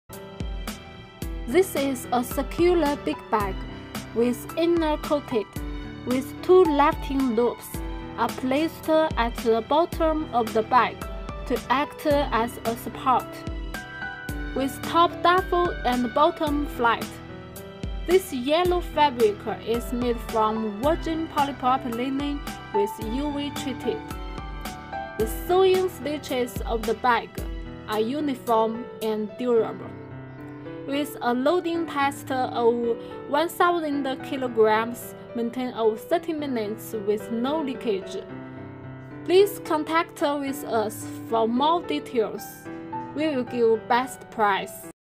FIBC big bag pp woven bag super sack 1000KG ton bags
This is a circular bigbag fibc supersack tonbag with inner coated, with two lifting loops are placed at the bottom of the bag to act as a support. With top duffle and bottom flat. This yellow fabric is made from virgin polypropylene with UV treated. The sewing stitches of the bag are uniform and durable. Loading Test of 1000KG maintain of 30minutes with no leakage of the contents or damage of the bag was observed.